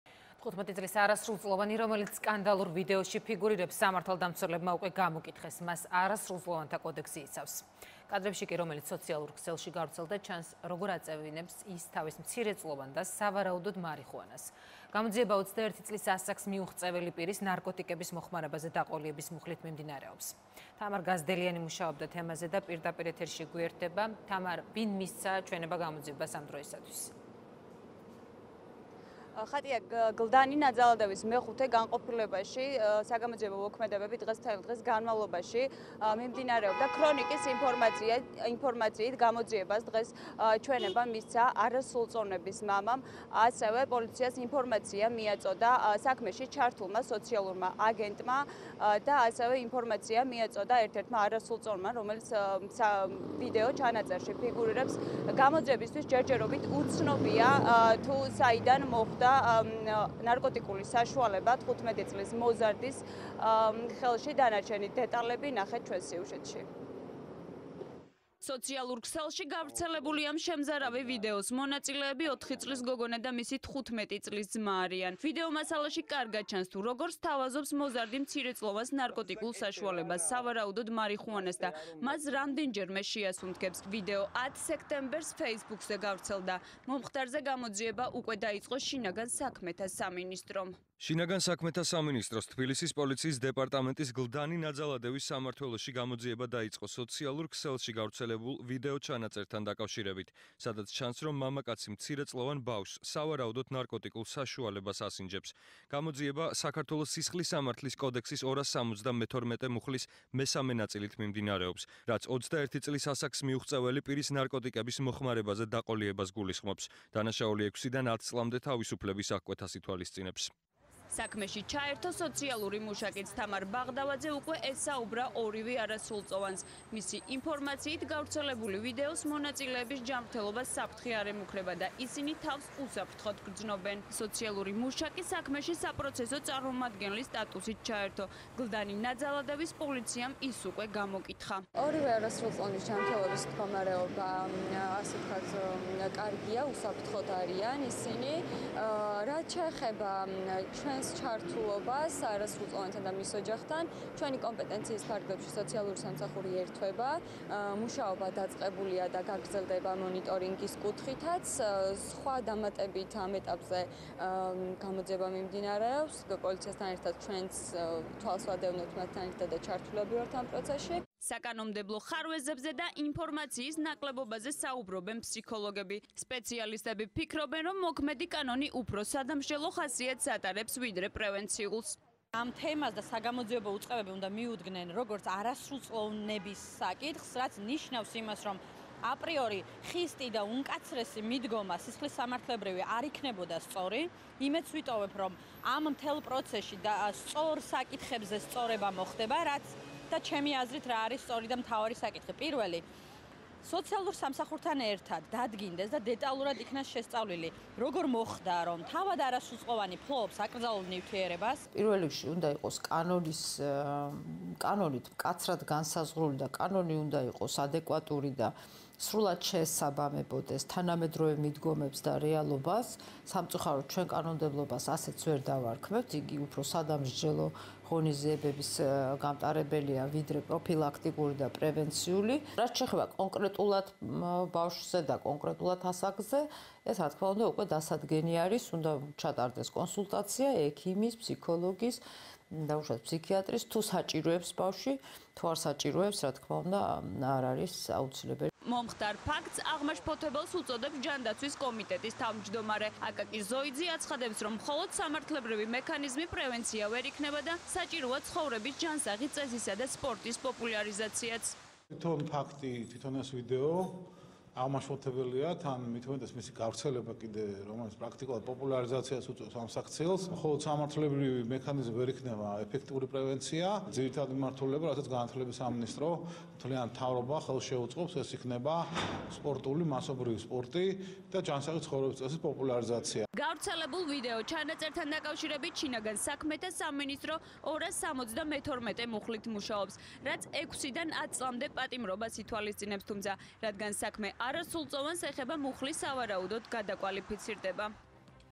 არასრულწლოვანი, რომელიც სკანდალურ ვიდეოში ფიგურირებს სამართალდამცველებმა გამოკითხეს
ყველა საიახლე იხილეთ ჩვენს ოფიციალურ ვებ-გვერდზე